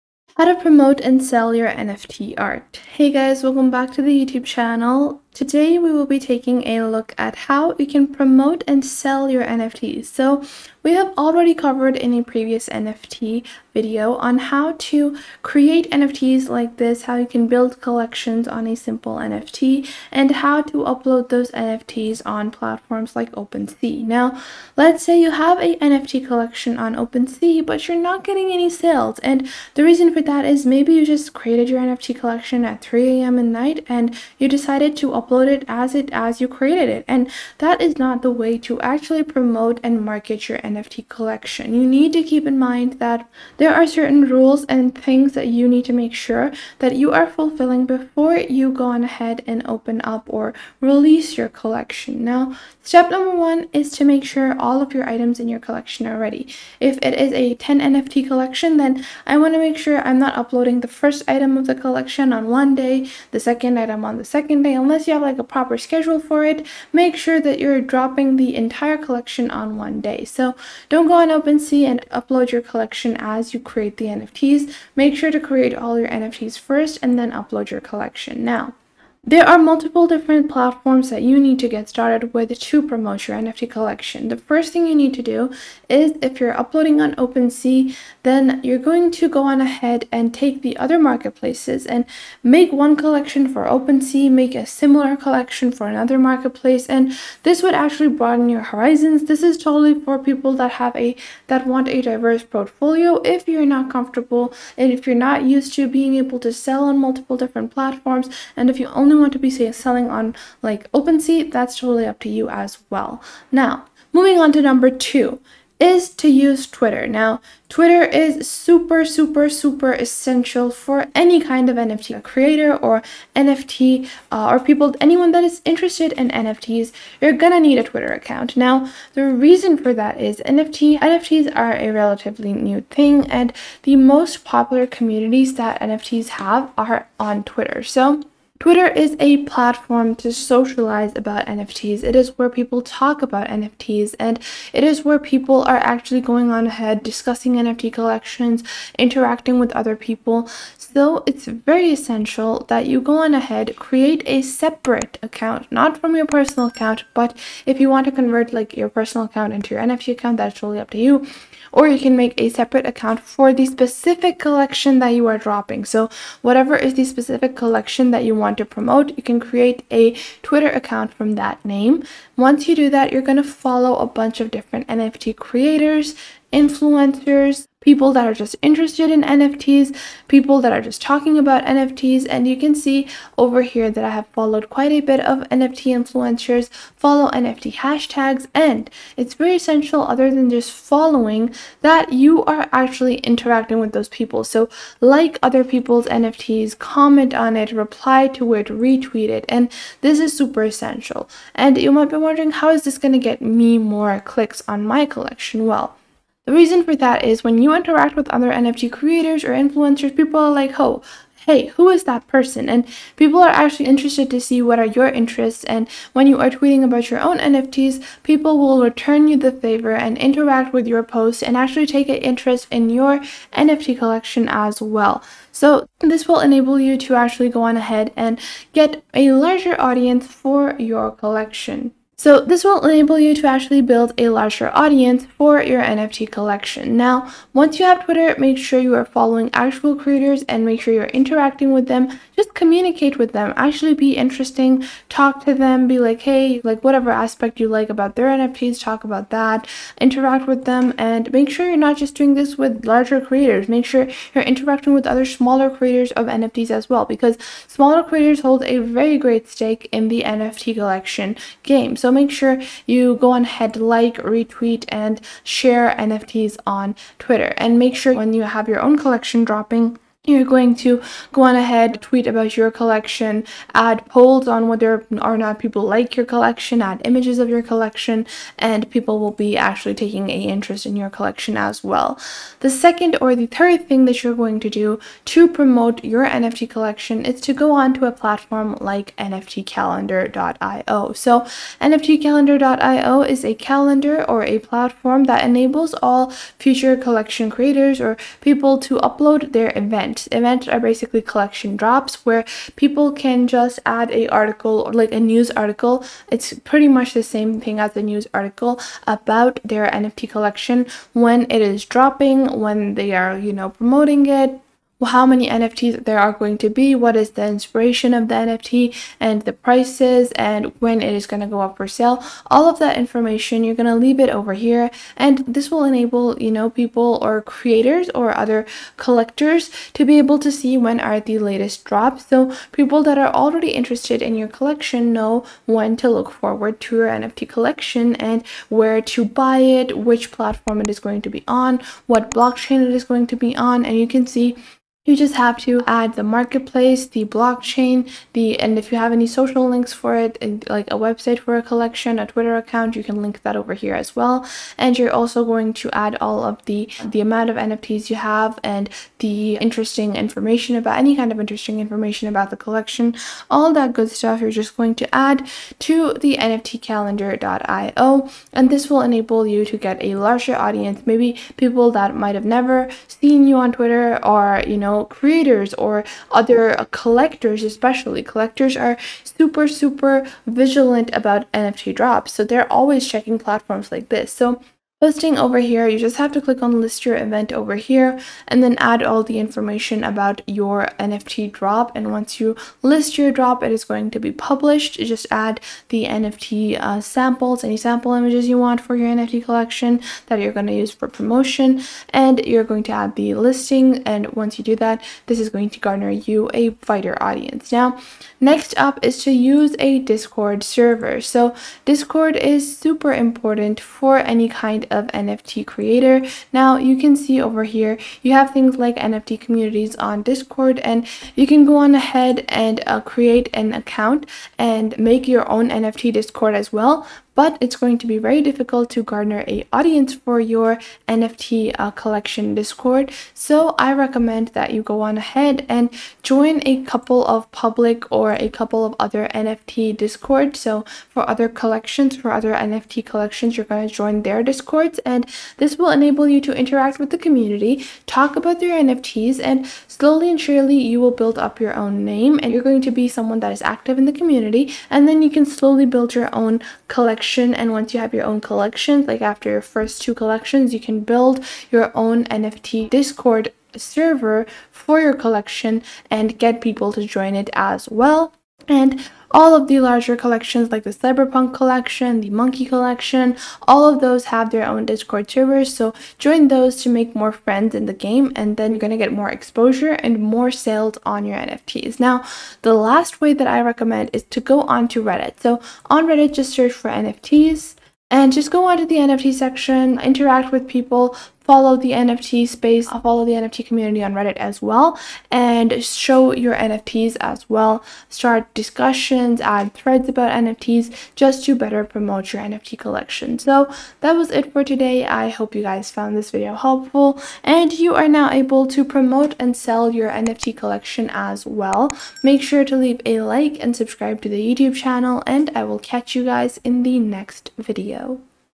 How To Promote And Sell Your NFT Art (2025)
In this video I show you How To Promote And Sell Your NFT Art. This is super easy and learn to do it in just a few minutes by following this helpful tutorial.